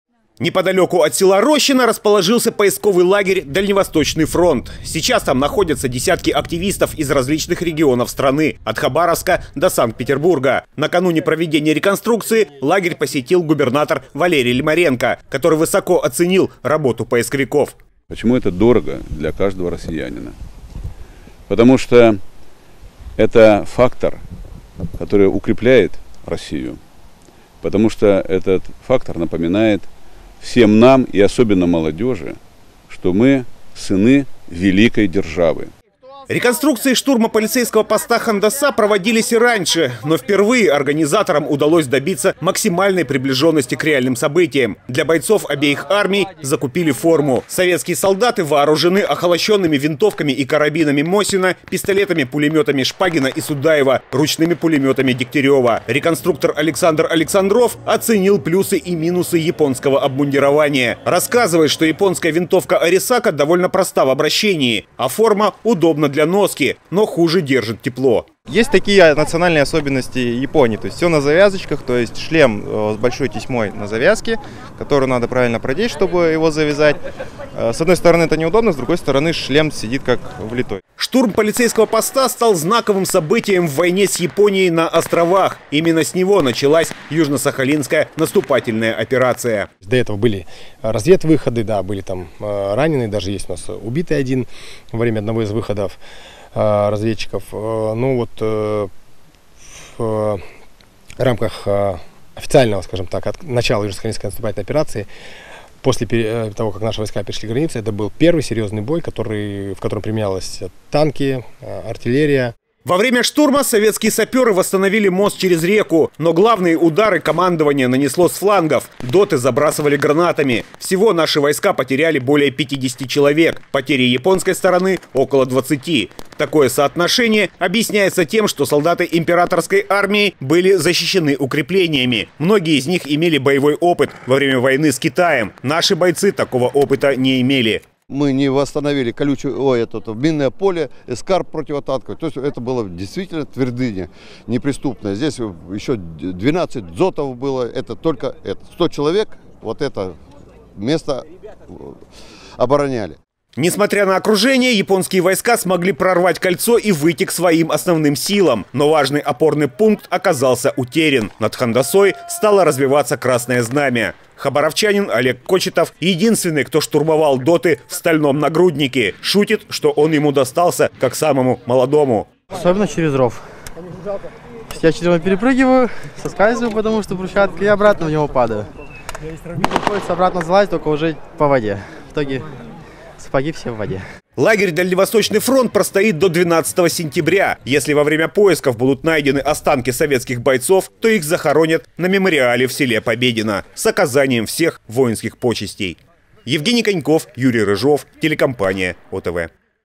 Бой за пост Хандаса. Историческая реконструкция в Смирных
Вперед за Родину. В селе Рощино прошла историческая реконструкция взятия полицейского поста Хандаса. В августе 45 года советские части перешли границу с Японией и начала освобождать Южный Сахалин. Бой за Хандасу стал первым серьезным столкновением с императорской армией. В реконструкции тех событий приняли участие более 70-ти человек. Все участники были одеты в форму тех лет и вооружены настоящими, хоть и охолощенными, винтовками и пулеметами.